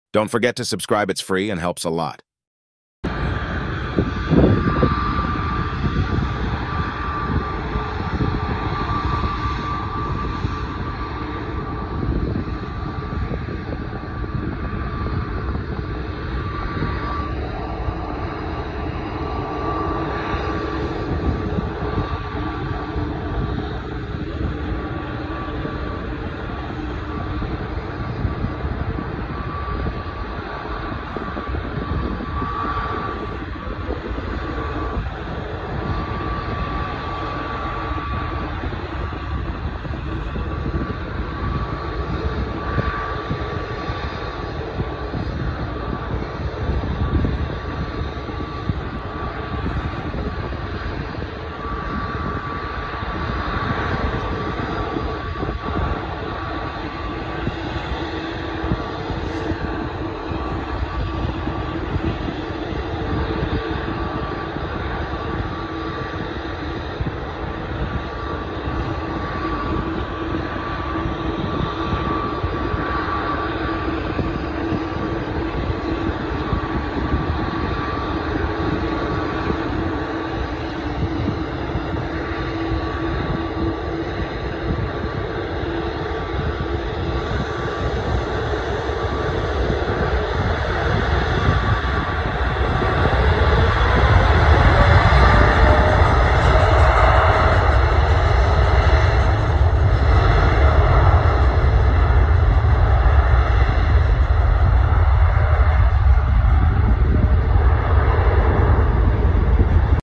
We LOVE a ETIHAD 787 Dreamliner Take Off! 💙✈️
Amazing motor sound  of Boeing 787 Dreamliner

It really helps us reach more avgeeks around the world. Thank You For Your Support!!